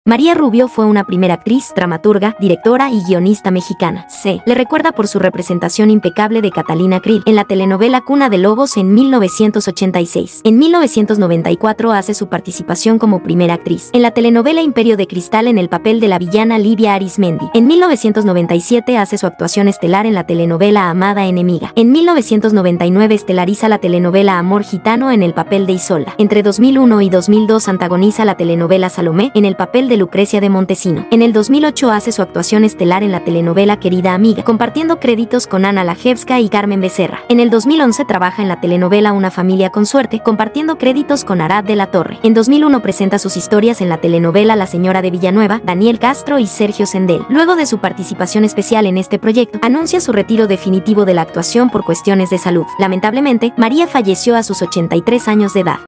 😲¡MARÍA RUBIO, ASÍ LUCÍA ANTES Y DESPUÉS!
Celebremos la vida y legado de María Rubio, la inolvidable Catalina Creel de Cuna de Lobos. Desde sus papeles icónicos hasta su retirada por motivos de salud, esta primera actriz dejó una huella imborrable en la pantalla. ¡Un emotivo viaje a través de su carrera y su impactante transformación!